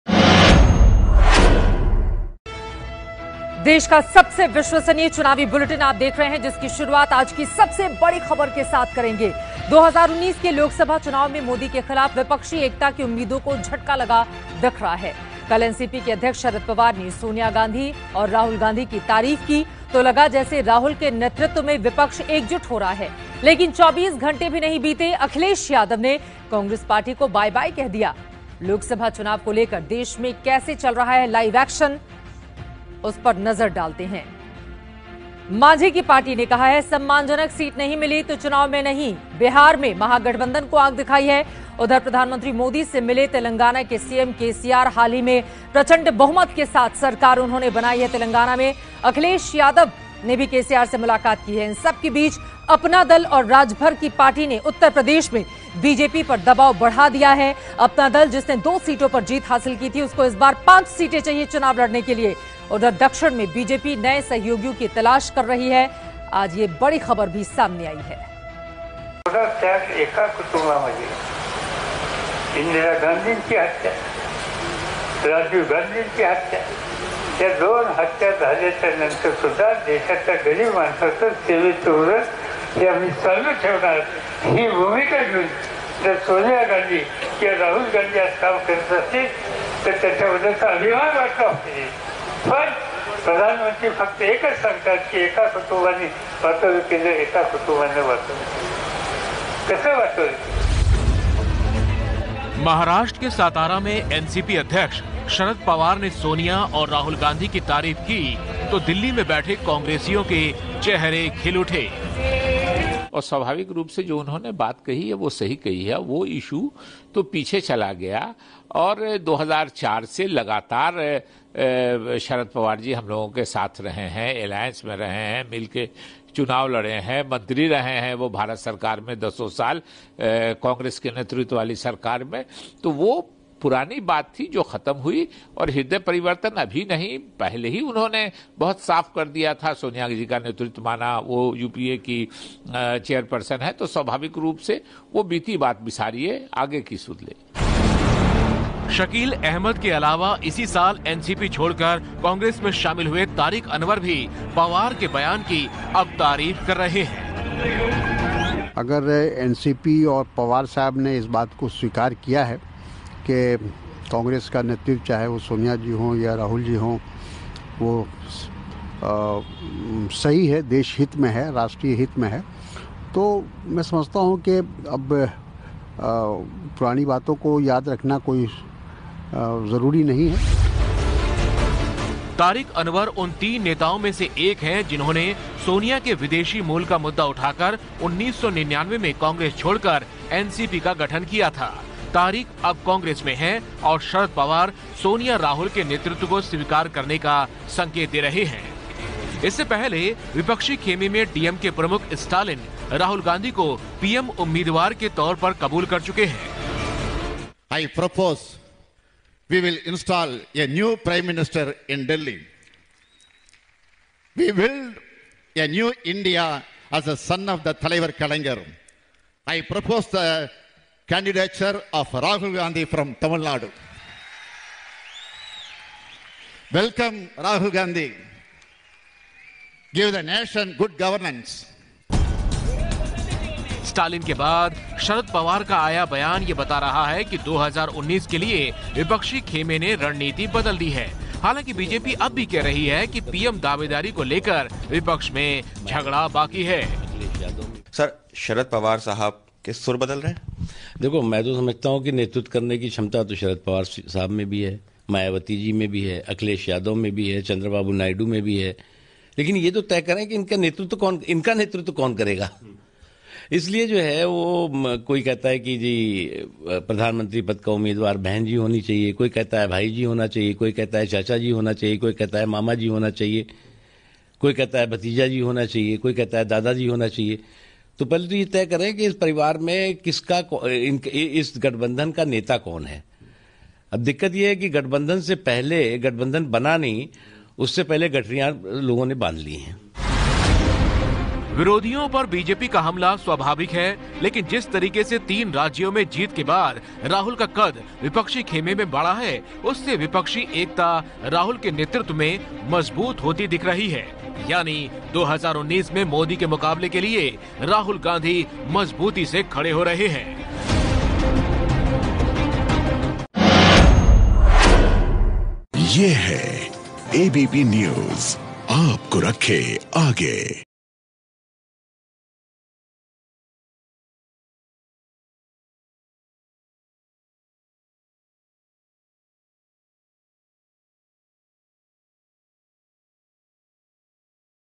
यूपी में अपना दल और राजभर की पार्टी ने बीजेपी पर बढ़ाया दबाव | ABP News Hindi
यूपी में अपना दल और राजभर की पार्टी ने बीजेपी पर बढ़ाया दबाव